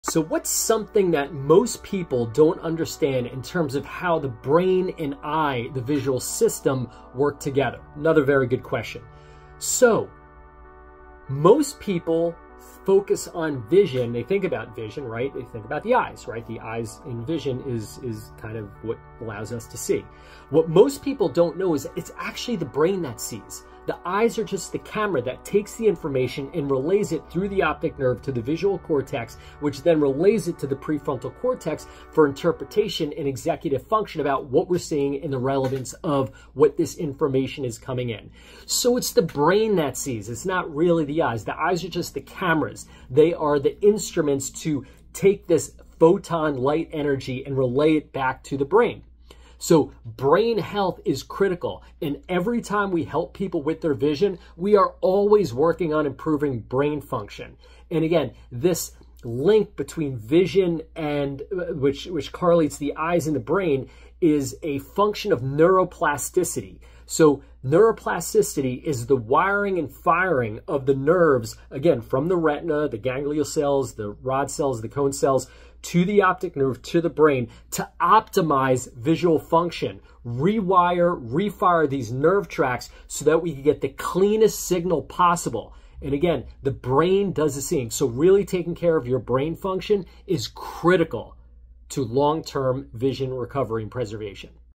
Did you know your eyes don’t actually see?
Most people think vision is all about the eyes, but the reality is… your brain is what truly sees. Your eyes are just the cameras capturing light and sending that information to your brain, which then processes and interprets what you’re looking at.

That’s why brain health is critical for your vision. Every time we work on improving eyesight, we’re also working on rewiring and strengthening the neural pathways between the eyes and the brain – a process called neuroplasticity.

✨ Healthy brain, better vision. Remember, it’s your brain doing the seeing – your eyes are just the instruments.